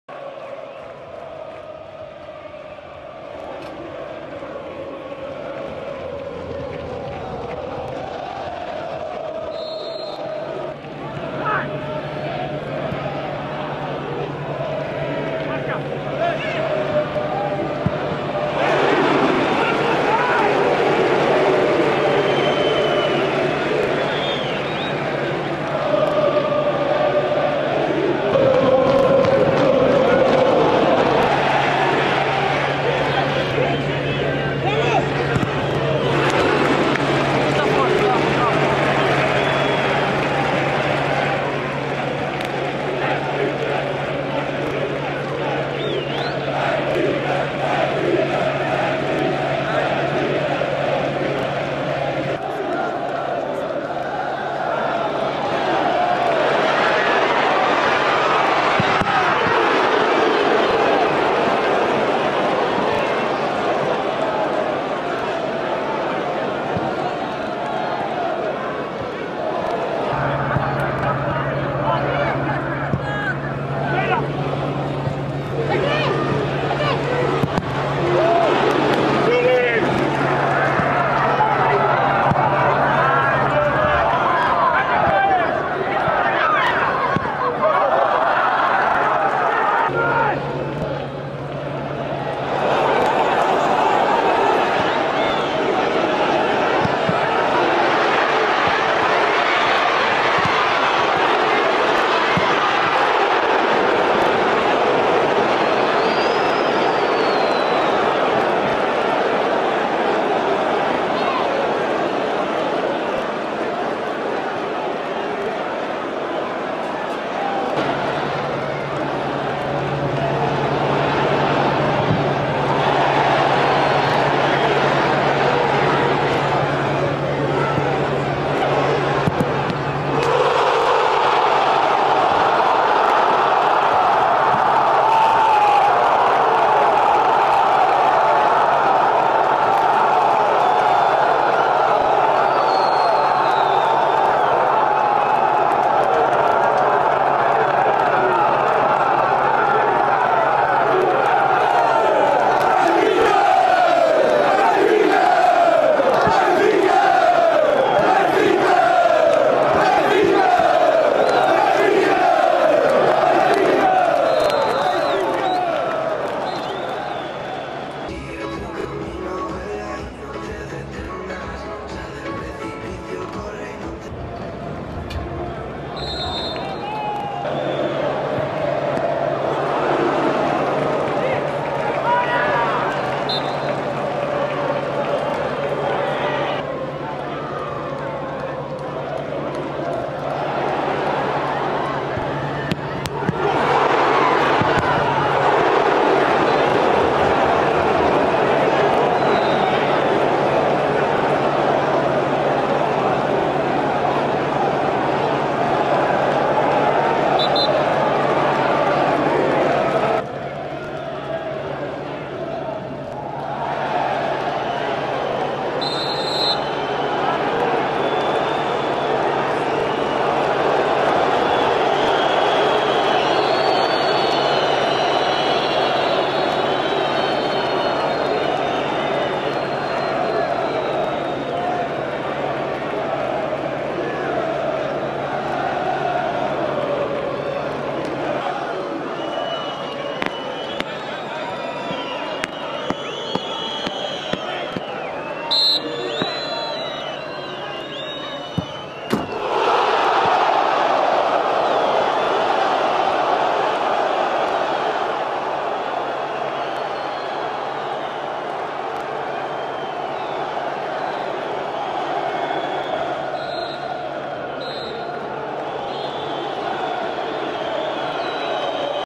Resumo: FC Vizela 0-2 Benfica - Liga Portugal bwin | SPORT TV
Termina a partida! Vitória suada do SL Benfica por 2-0 no terreno do FC Vizela, em jogo da 22.ª jornada da Liga Bwin. Um bis de João Mário, que abriu o marcador aos 38 minutos e já nos descontos converteu uma grande penalidade. A equipa vizelense até dispôs de oportunidades para marcar mas não foi eficaz na finalização.